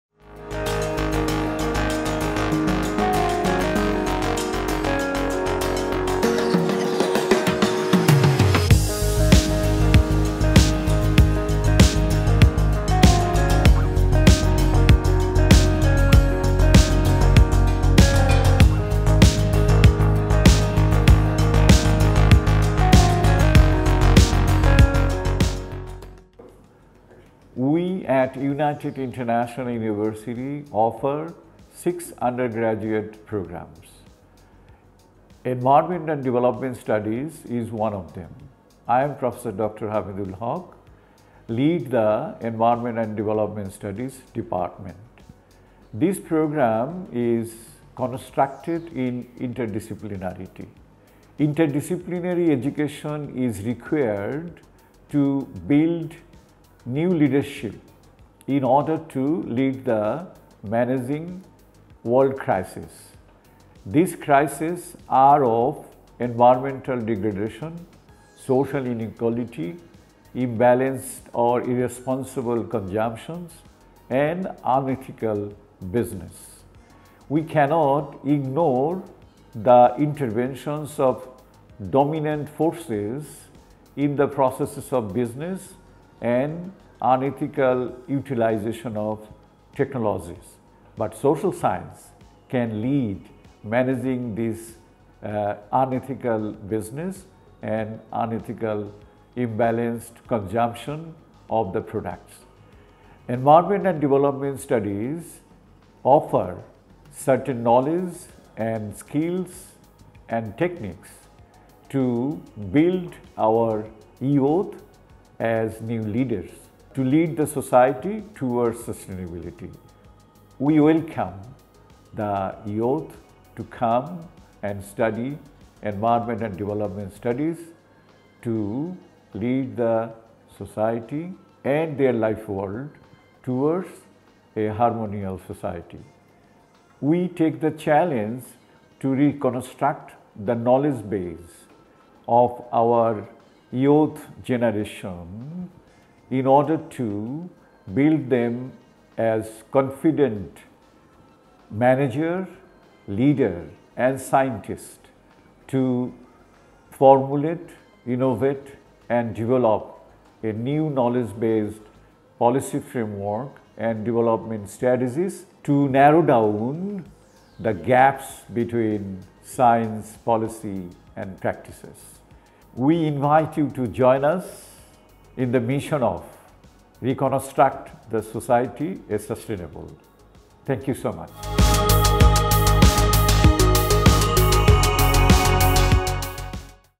Department of Environment and Development Studies I UIU
Sustainable Development is a challenge of the moment. The youth are expected to lead the sustainable development movement. There is no alternative of interdisciplinary knowledge and skills in leading Sustainable Development. Understanding empirically the dynamics of three main ‘Pillars of Sustainable Development: Environment, Social, and Economic Development’ is crucial. The UIU offered BSS (Hons.) in Environment and Development Studies builds you as knowledgeable and skilled young leader to lead the reconstruction of a knowledge-based sustainable society. The graduates in Environment and Development Studies from UIU will have employment opportunities in the government organizations, banks and financial institutions, education and research institutions, non-government and private organizations, NGOs, INGOs, industries, electronic and print media, power sector organizations, and in various sectors in national and international domain.

In a speech in 2015, Mr. Bill Gates, co-founder of the Microsoft Corporation, predicted about an infectious virus and an impending epidemic, coinciding with the current global epidemic of corona virus infection. Few months ago, he predicted another catastrophic pandemic that is ‘Climate Change’. By 2060, climate change will be as deadly as Covid-19 and by 2100, it will be five times more frightening than the current one.

To win over the challenges the world facing today related to climate change and sustainable development, there is no substitute of having up-to-date knowledge of environment and development. UIU’s BSS in Environment and Development Studies promises to build leaders in national and international spheres in tackling climate change catastrophes, which is a barrier to sustainable development.

Special features of BSS (Hons.) in Environment and Development Studies program:

Interdisciplinary knowledge of environment and development
Develop knowledge and skills through active learning
Strong focus on practical study to enhance research skills
Scope of Residential Field Study/partial semester work
Well-equipped Lab and Library facilities
Opportunity to become academician, scientist or professional
Up to 100% Scholarship opportunity
Opportunity to participate in the National and International Conferences
Opportunity to take part in the various national and international seminars, workshops and trainings.